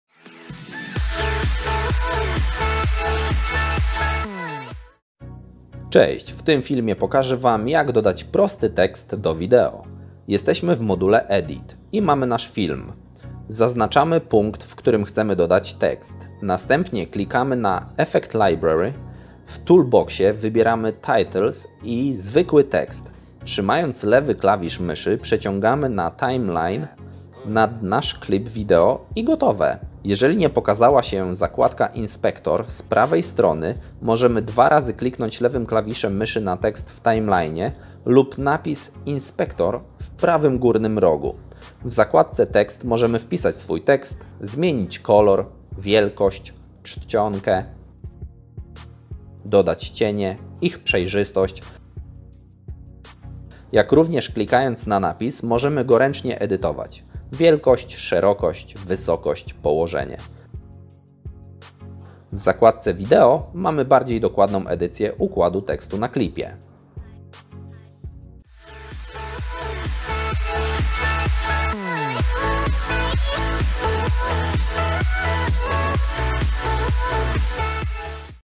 Davinci Resolve 16 - #2 Jak dodać prosty tekst do wideo w 1 minutę!
Witam
W tym filmie pokaże Wam jak dodać prosty efekt tekstowy do naszego wideo.

W razie pytań proszę o komentarz. Jeżeli będę wiedział to odpowiem na pytanie :)
Z góry przepraszam za moją angielską wymowę :)